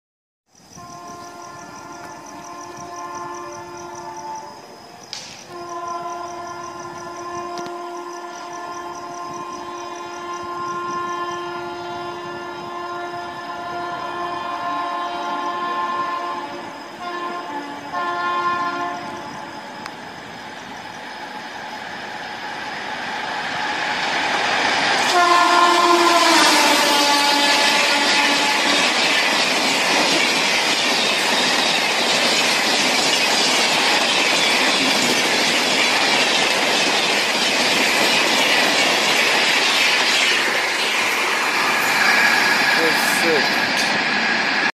Sealdah Rajdhani Express sasaram
Presenting sealdah rajdhani, the real night rider.
Blowing away everything at sasaram jn (Bihar).
On that day I'm waiting for kolkata mail at sasaram jn. Just because my train was late. I get chance for capture my fav train at bihar. When i saw the signal went green, 10 min before its crossing. I'm sure that this train must be show a aggressive show. And my words come true. Just enjoy honking and speed of rajdhani  express.